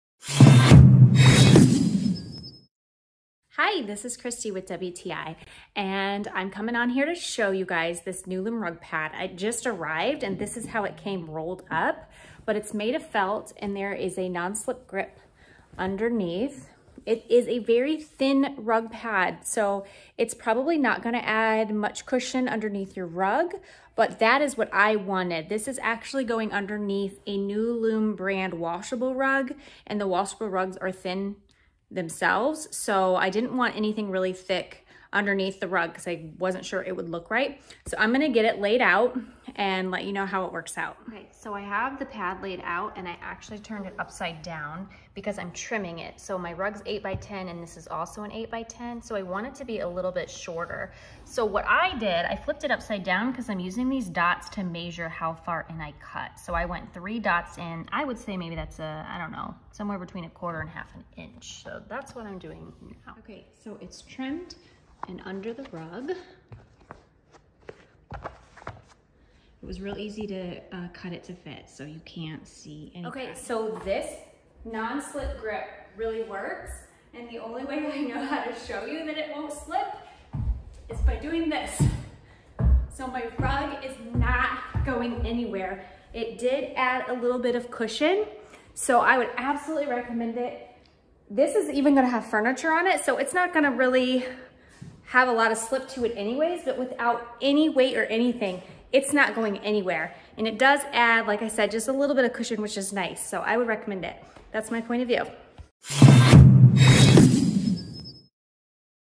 Our Point of View on nuLOOM Non-Slip Felt Rug Pads From Amazon
(Commissionable Links)

about this PRODUCT:
100% Recycled Synthetic Fibers
Material: 100% Recycled Synthetic Fibers
Protects hardwood floors and premium tile from friction cause by foot traffic
Add plushness to your rug while keeping it securely in place
Can be easily trimmed to fit any size rug. Moisture Barrier-No